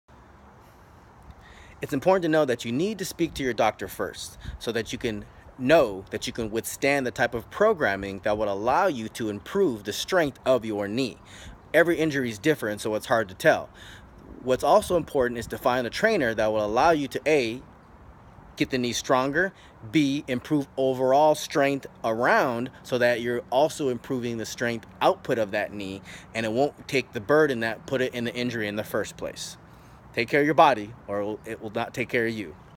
Daniel J Sanidad | How to recover from a knee injury?
How to recover from a knee injury?

See your doctor to  ensure you have followed
the protocols allowing you to withstand the necessary programming to improve from your injury.

Daniel J Sanidad
Personal Trainer
In an industry inundated with misinformation, trend, and ineffective methods, I bring efficiency, sustainability, and practical strategy back to exercise.